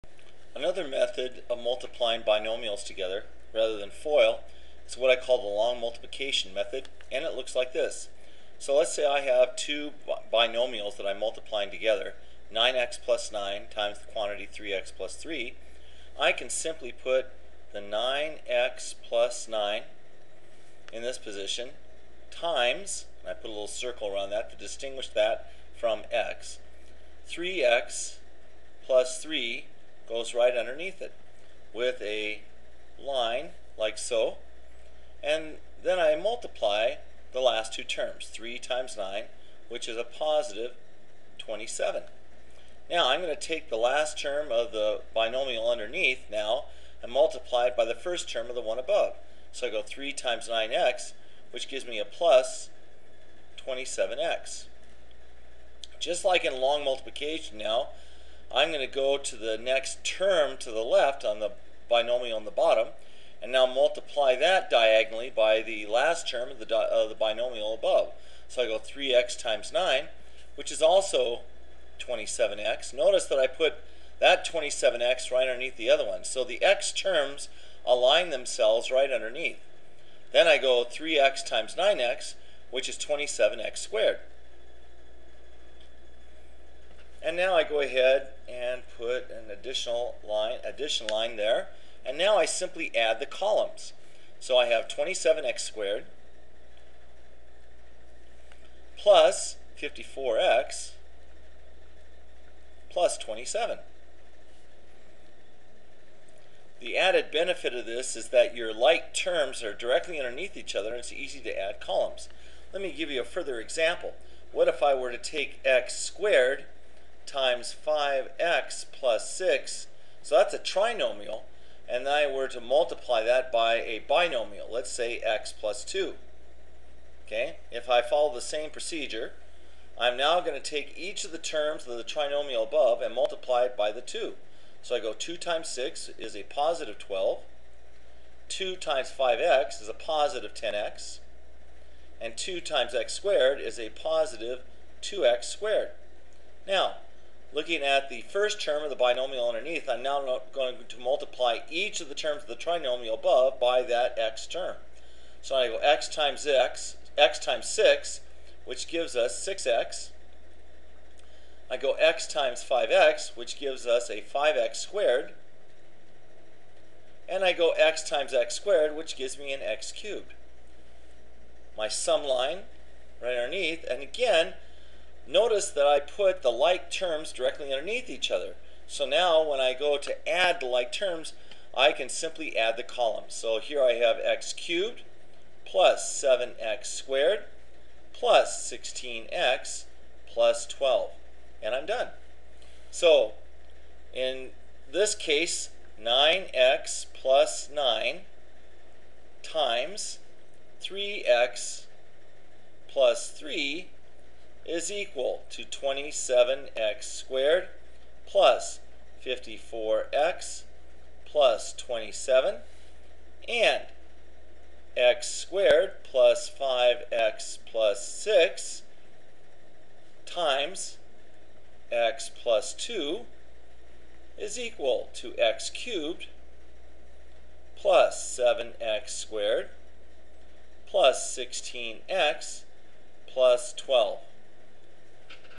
Binomial Multiplication Vertical Method.avi
Multiplying polynomials by long multiplication protocal. Good process for multiplying polynomials other than binomials as well.